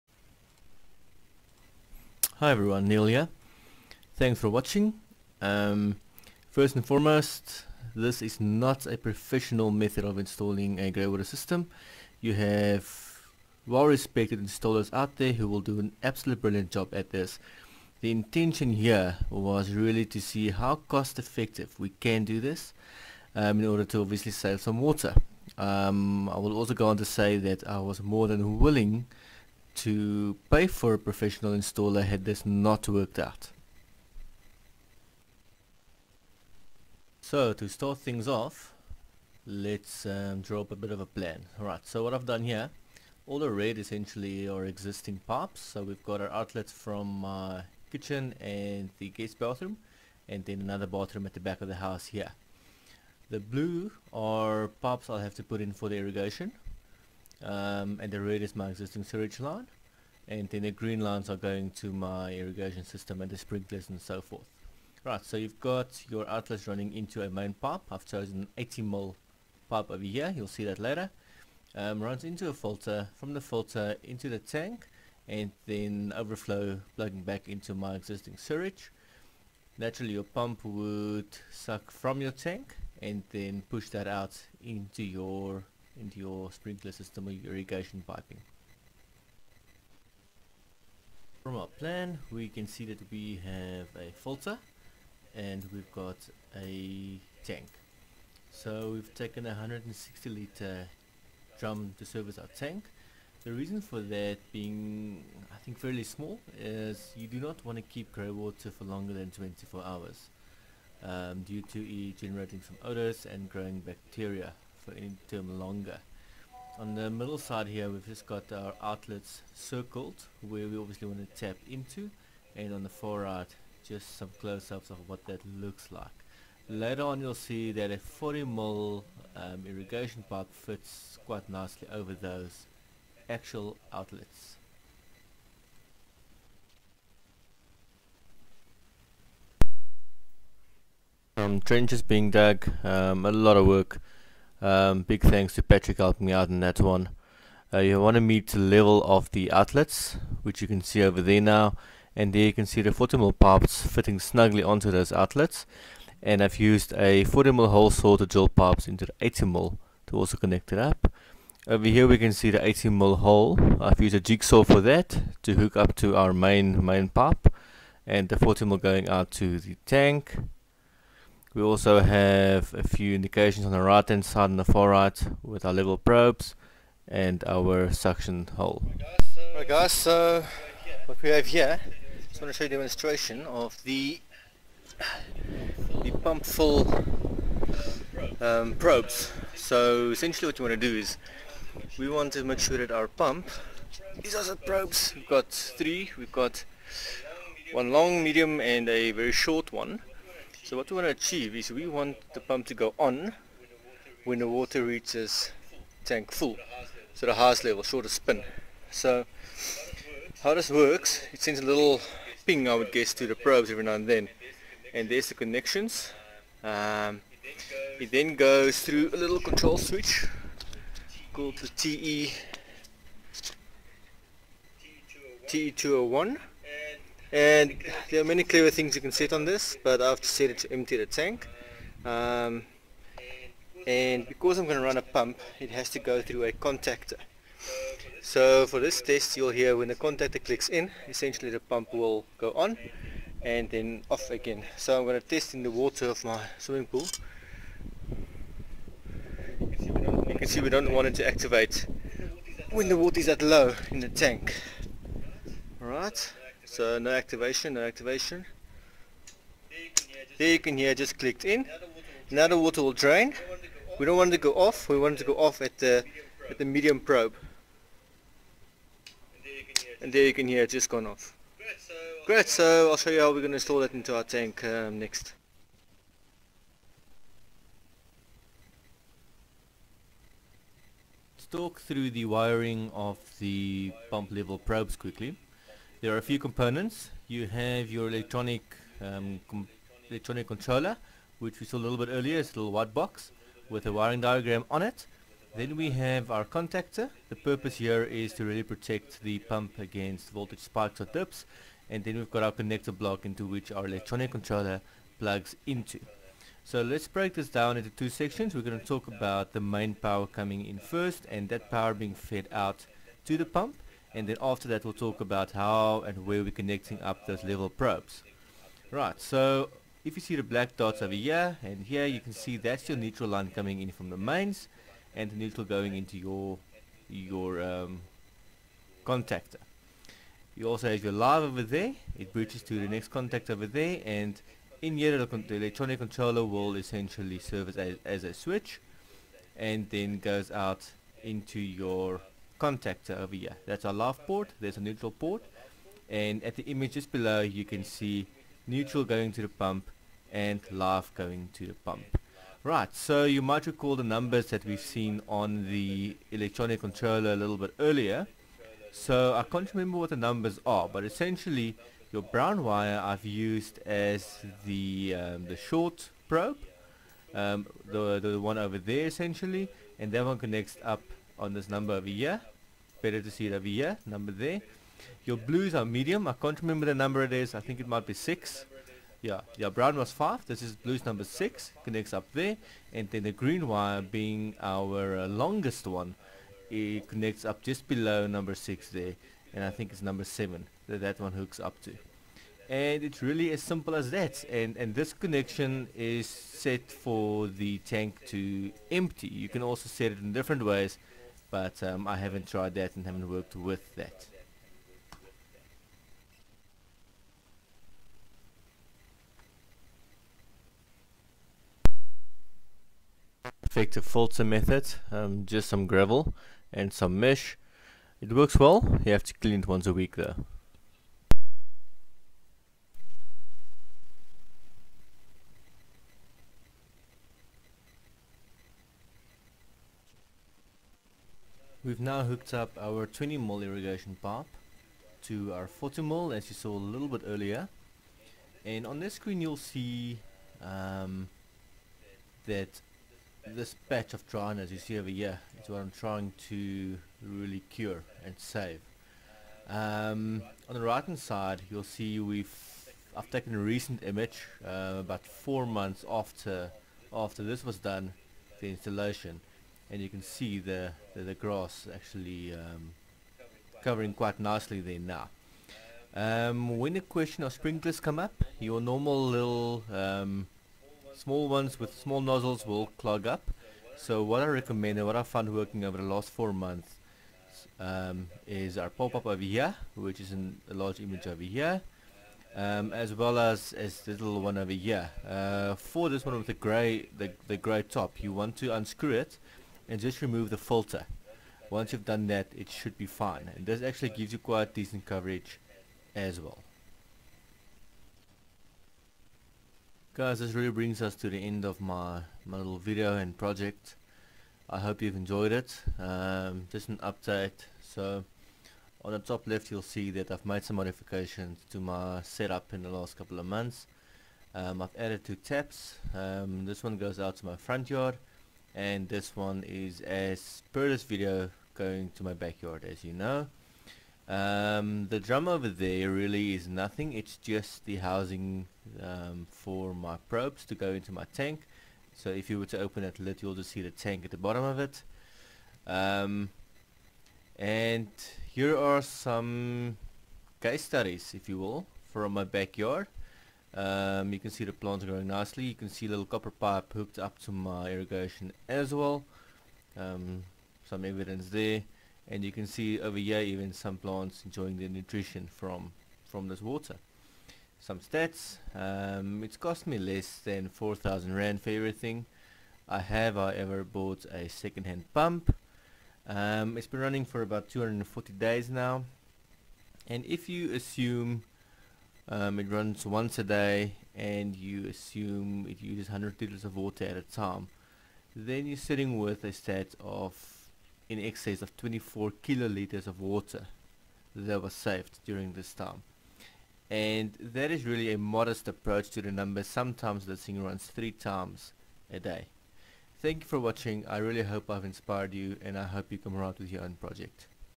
DIY Grey water system on a budget
Build and install your own grey water system at home and hook it up to your existing irrigation.